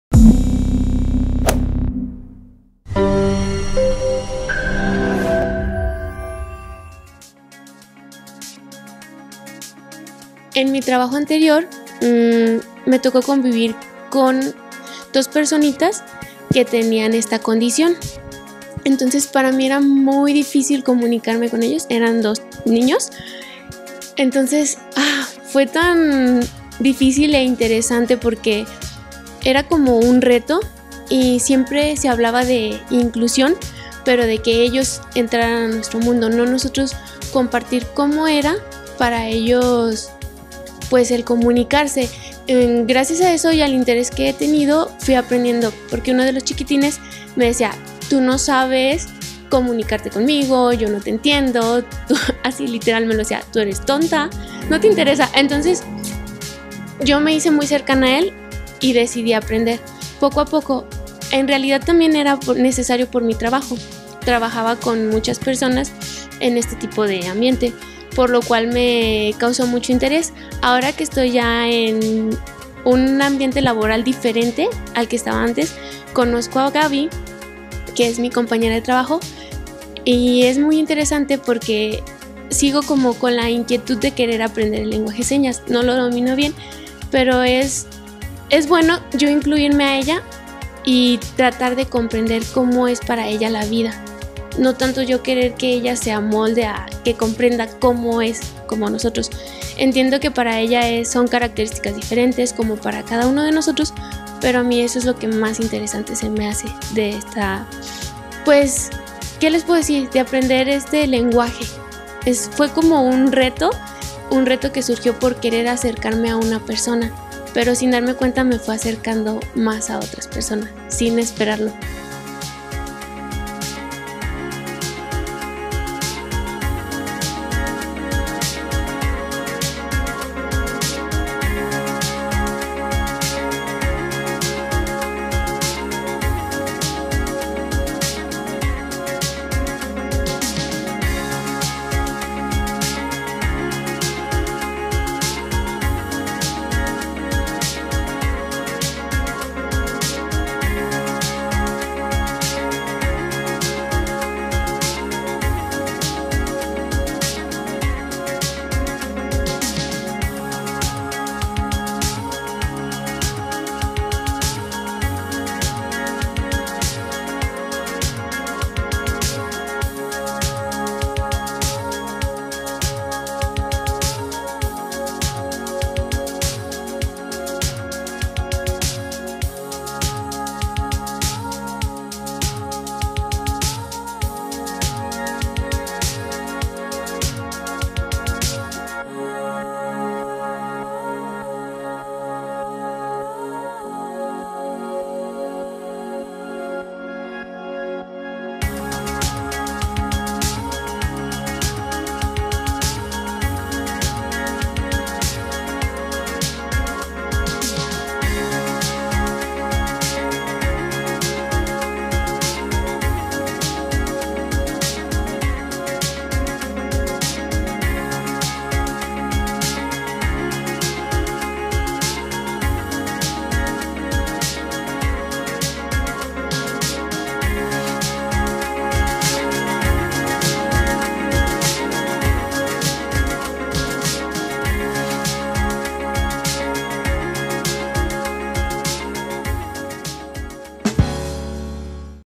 Mi vida siendo madre con discapacidad
En el Día Internacional de la Sordera, Gaby nos comparte un poco de su vida. Descubre cómo es vivir Día a día con una persona con discapacidad”